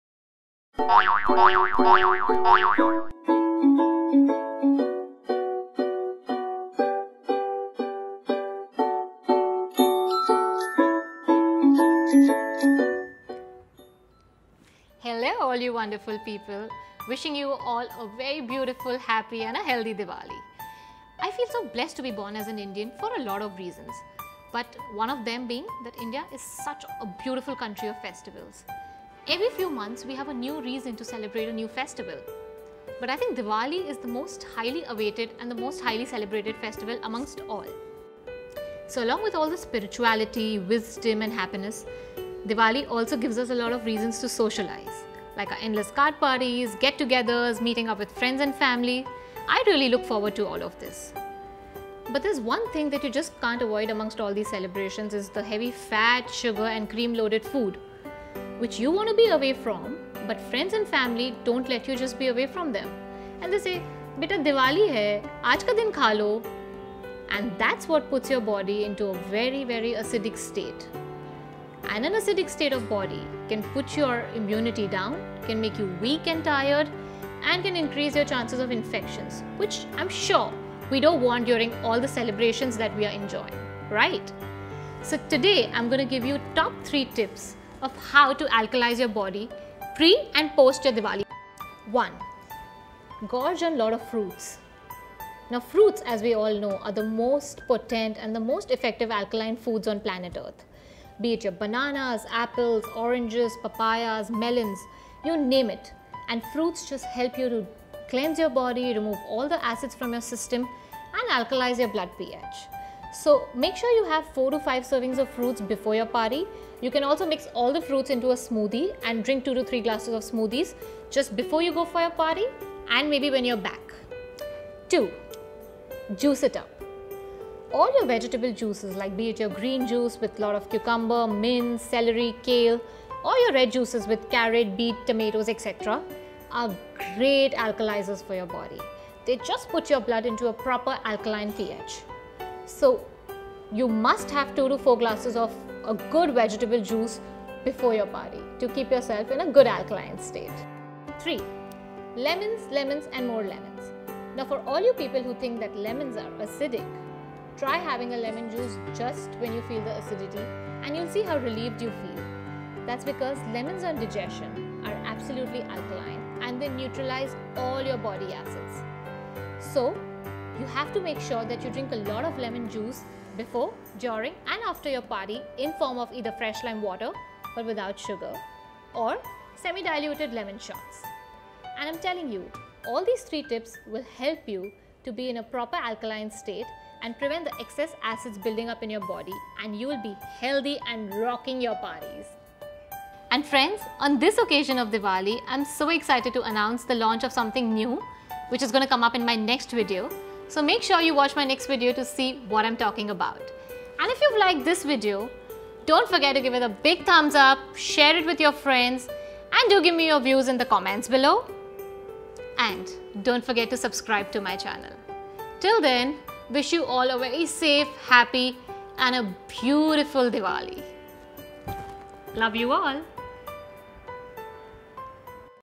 Stay healthy this diwali!!
Diwali is a beautiful festival with a lot of positivity and happiness all around. The whole aura diwali brings around a lot of reasons to celebrate..but staying healthy to enjoy the festival is the most important thing.. So for all my friends who want to look and feel awesome this diwali.. This one is for you !!

Happy Diwali

Jewellery courtesy: The gorgeous jewellery am wearing in this video is my cutest friend Rashi Rochwani..

And the most important person behind this video:
My hubby dearest Riki Ranglani

LOVE
XX
NEHA